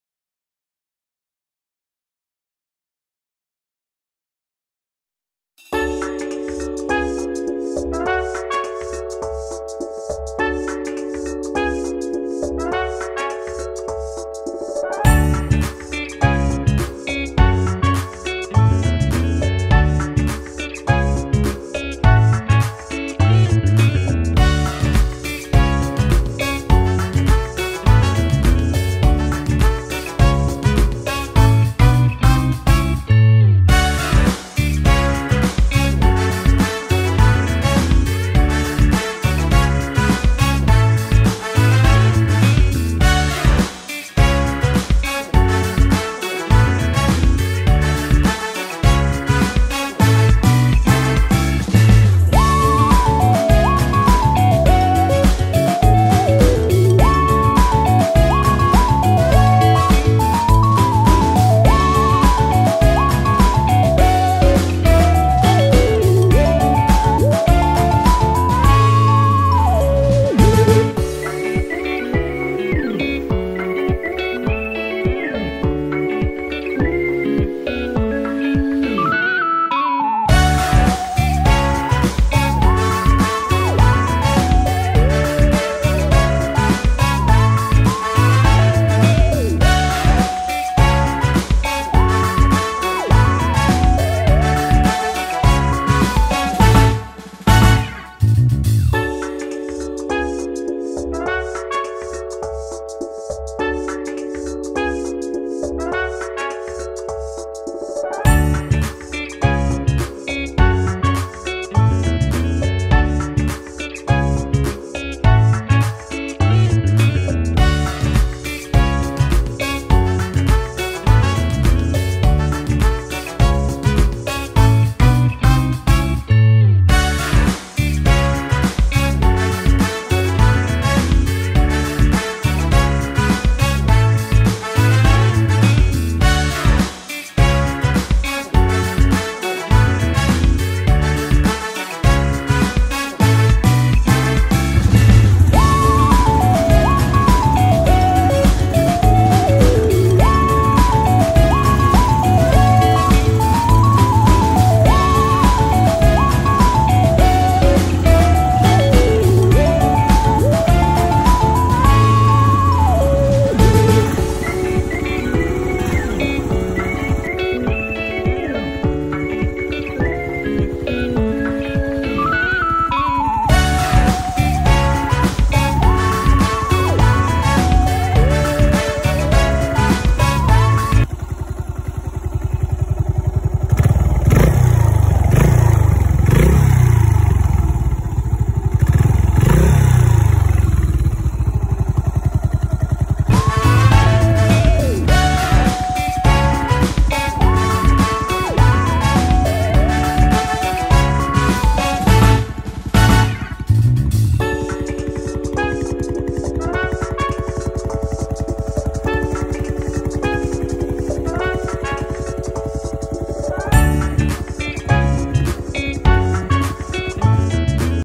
ヤマハSR400 インジェクションモデル#趣味のバイクカスタム🏍️
シングル単気筒の鼓動とアナログカスタムの可能性
を秘めた貴重なバイクです。